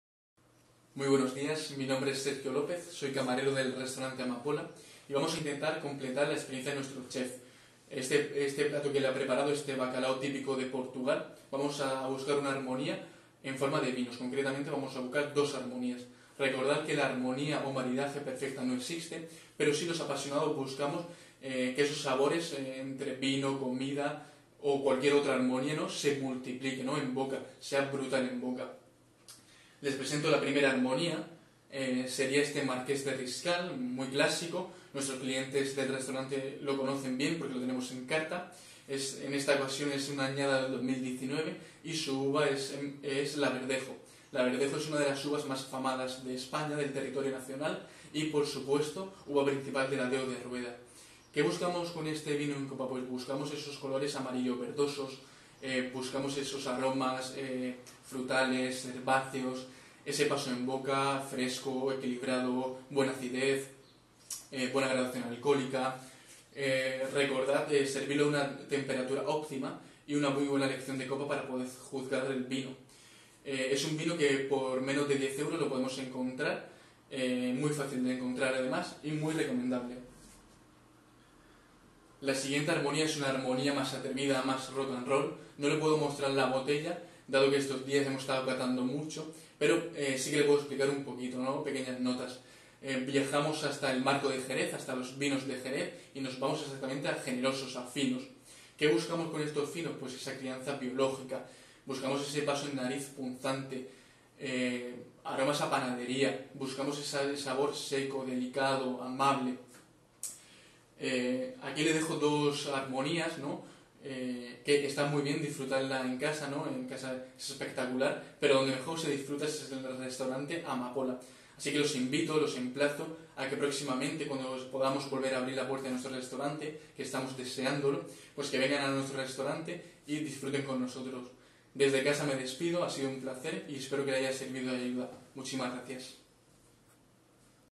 Experiencia de Maridaje - Wine Pairing Experience- Sergio López - Amapola restaurant
🇬🇧 Following the Cook at Home experience.

Our waiter, Sergio López, from Amapola restaurant is presenting us an interesting wine pairing option for the previous Portuguese dish, Bacalhau a Bràs. We hope you like it and you can enjoy an even better experience on your next visit to Amapola restaurant.

🇪🇸 Siguiendo la video receta de Cocina en Casa.

Nuestro camarero, Sergio López, del restaurante Amapola nos presenta una interesante opción de maridaje de vinos para el plato portugués anterior, Bacalao a Bràs. Esperamos que les guste y puedan disfrutar de una experiencia aún mejor en su próxima visita al restaurante Amapola.